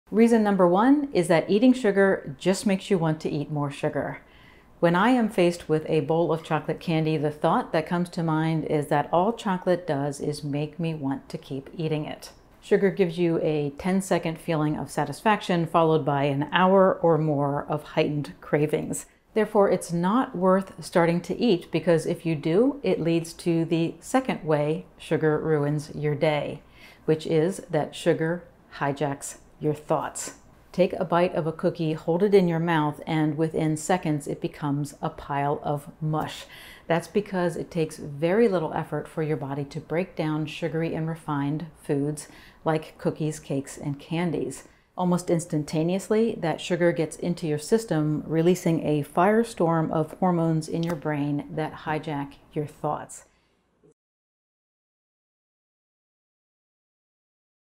6 Reasons Not to Eat Sugar - Reasons 1 & 2
Science has made it clear that eating sugar leads to poor health. But sugar tastes good, is in seemingly everything, and is pretty cheap. On top of that, poor health is a future thing, making it hard to come up with a reason to put down the cookie or avoid grabbing the candy bar. Here are two of the six less-scientific, but highly motivating reasons you don’t want to ruin your day by eating sugar.